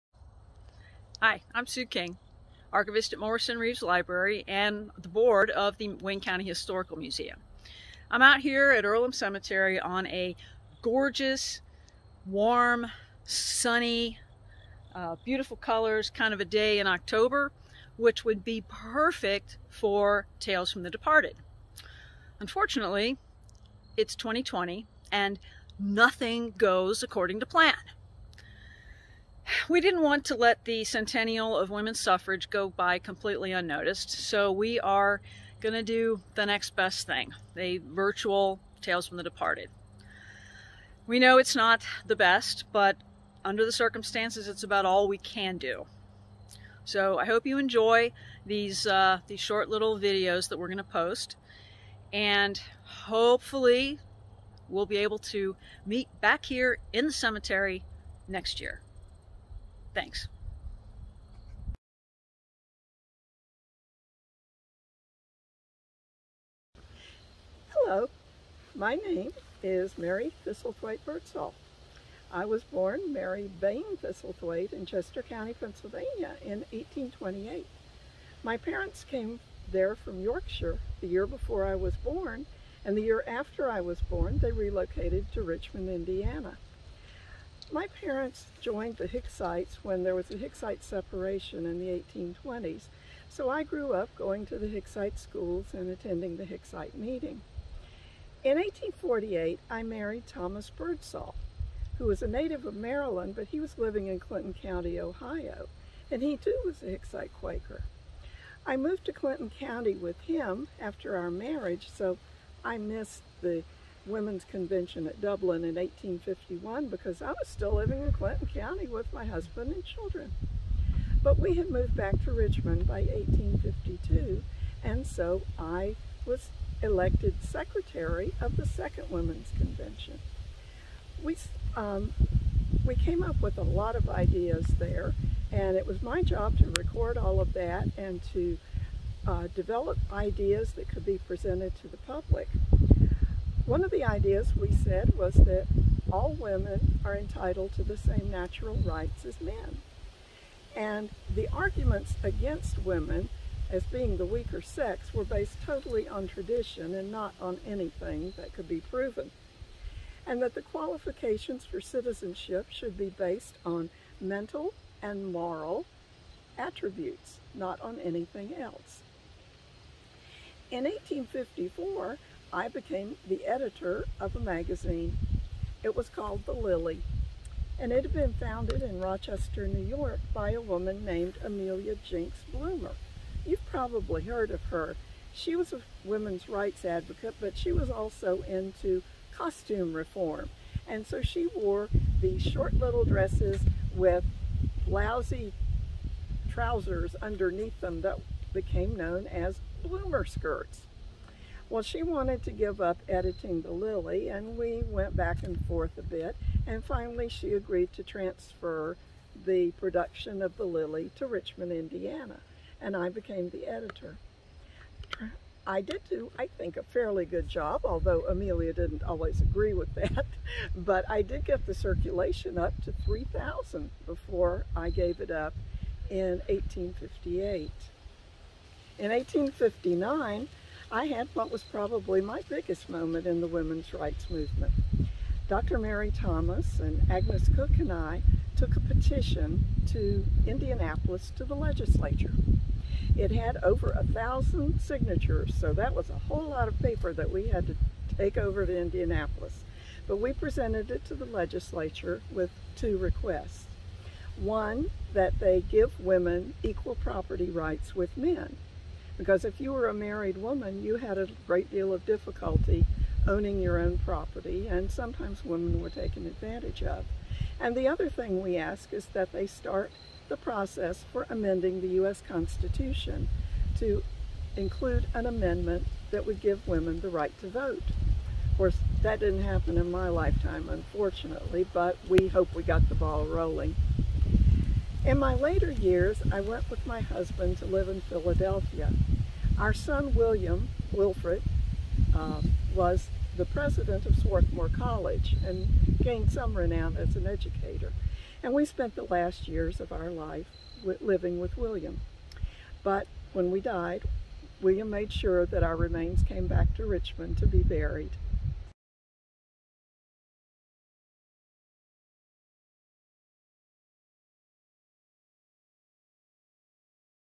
Tales From the Departed: Mary Birdsall
Our Virtual Tales From the Departed Continues in this video on Mary Birdsall! Enjoy!